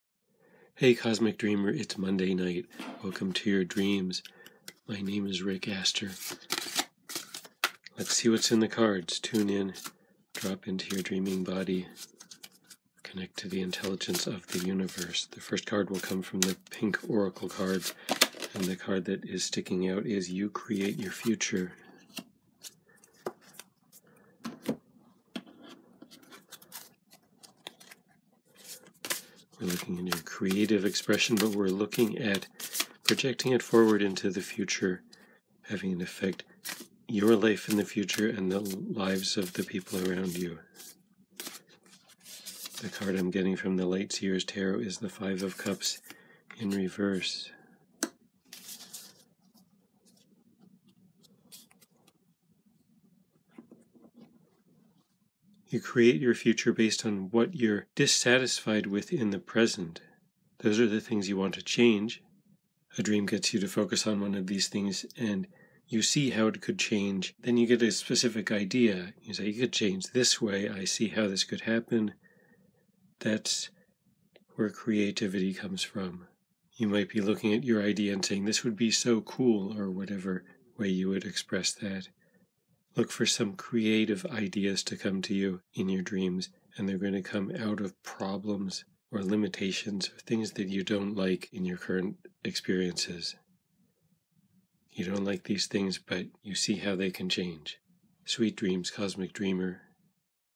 Dreams 2023-02-06 🎨 A New Creative Idea
Oracle reading with Rick Aster for your dreams tonight. Tune in to the ideas and energies of expansion before you go to sleep in order to connect to your dreaming body and the intelligence of the universe.